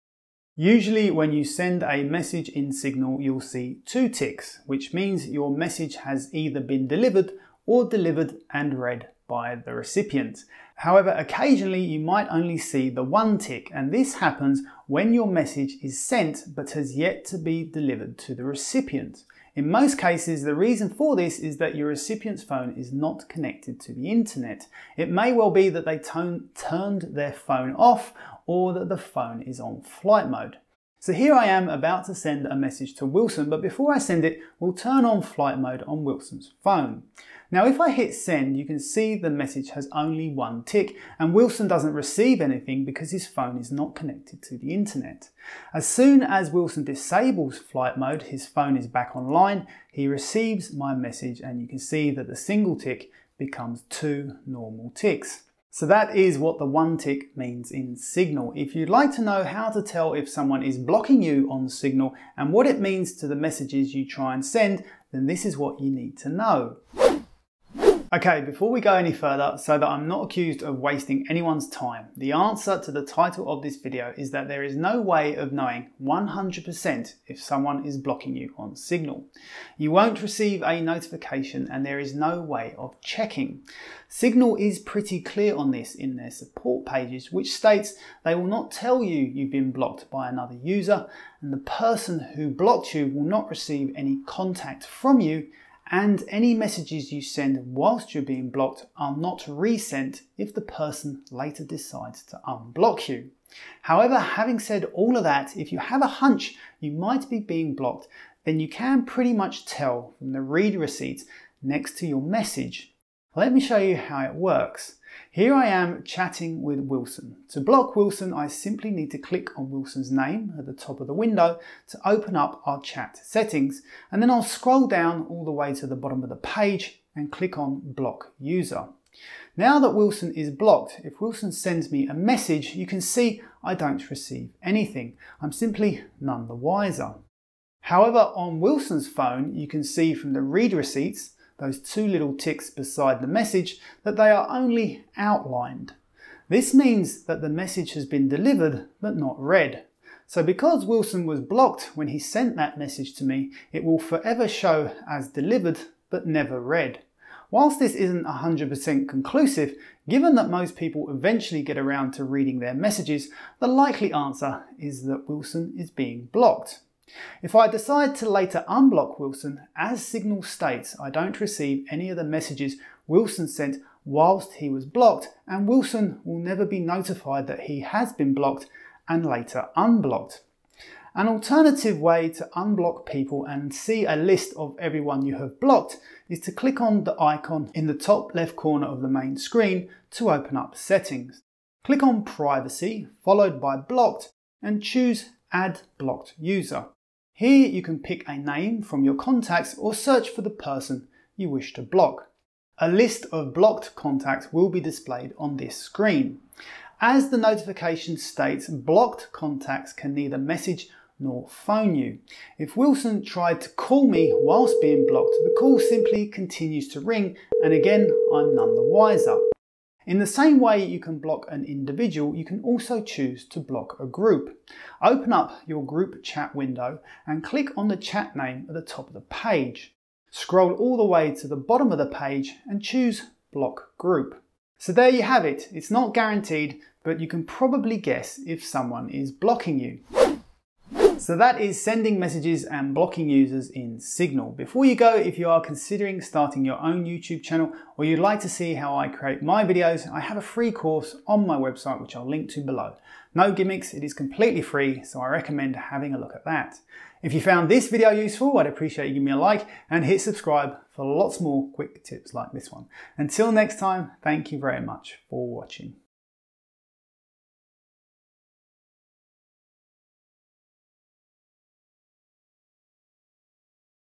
Only One Check Mark On Signal - What Does It Mean?
One checkmark or tick in Signal occurs when your message is sent but yet to be delivered to the recipient's phone. In most cases, the reason for this is that the recipient's phone is not connected to the internet. The phone may well be turned off or on flight mode.

0:00 - What two ticks mean on Signal.
0:10 - What one tick means in Signal.
0:19 - The reason for only one checkmark on Signal.
0:30 - How one checkmark works on Signal.
0:58 - How to tell if someone is blocking you on Signal.